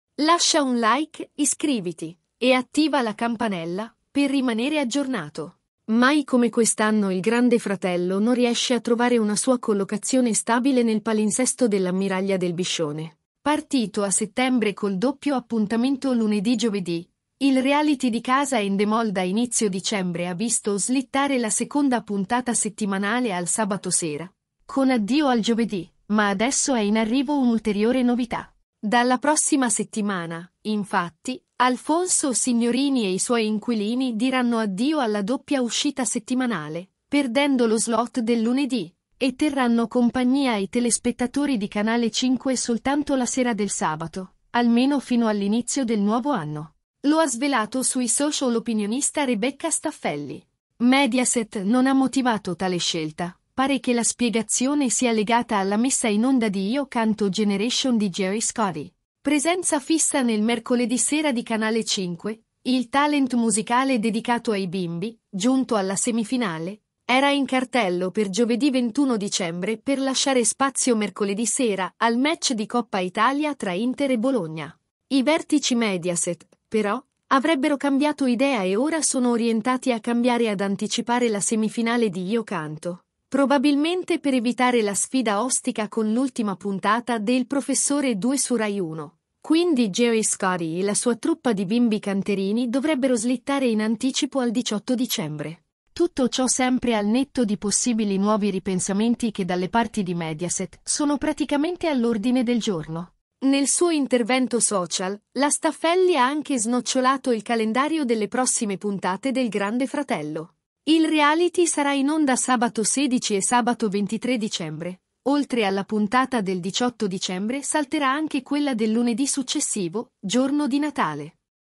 GRANDE FRATELLO salta la diretta
Mai come quest’anno il Grande Fratello non riesce a trovare una sua collocazione stabile nel palinsesto dell’ammiraglia del Biscione. Partito a settembre col doppio appuntamento lunedì/giovedì, il reality di casa Endemol da inizio dicembre ha visto slittare la seconda puntata settimanale al sabato sera (con addio al giovedì), ma adesso è in arrivo un’ulteriore novità. Dalla prossima settimana, infatti, Alfonso Signorini e i suoi inquilini diranno addio alla doppia uscita settimanale – perdendo lo slot del lunedì – e terranno compagnia ai telespettatori di Canale 5 soltanto la sera del sabato, almeno fino all’inizio del nuovo anno. Lo ha svelato ieri sera sui social l’opinionista Rebecca Staffelli. Mediaset non ha motivato tale scelta, ma stando a un retroscena pubblicato sul blog Davidemaggio pare che la spiegazione sia legata alla messa in onda di Io Canto Generation di Gerry Scotti. Presenza fissa nel mercoledì sera di Canale 5, il talent musicale dedicato ai bimbi, giunto alla semifinale, era in cartello per giovedì 21 dicembre per lasciare spazio mercoledì sera al match di Coppa Italia tra Inter e Bologna (valido per gli ottavi di finale). I vertici Mediaset, però, avrebbero cambiato idea e ora sono orientati a cambiare ad anticipare la semifinale di Io Canto, probabilmente per evitare la sfida ostica con l’ultima puntata de Il Professore 2 su Rai 1, quindi Gerry Scotti e la sua truppa di bimbi canterini dovrebbero slittare in anticipo al 18 dicembre. Tutto ciò sempre al netto di possibili nuovi ripensamenti che dalle parti di Mediaset – soprattutto negli ultimi mesi – sono praticamente all’ordine del giorno. Nel suo intervento social, la Staffelli ha anche snocciolato il calendario delle prossime puntate del Grande Fratello. Il reality sarà in onda sabato 16 e sabato 23 dicembre; oltre alla puntata del 18 dicembre salterà anche quella del lunedì successivo, giorno di Natale.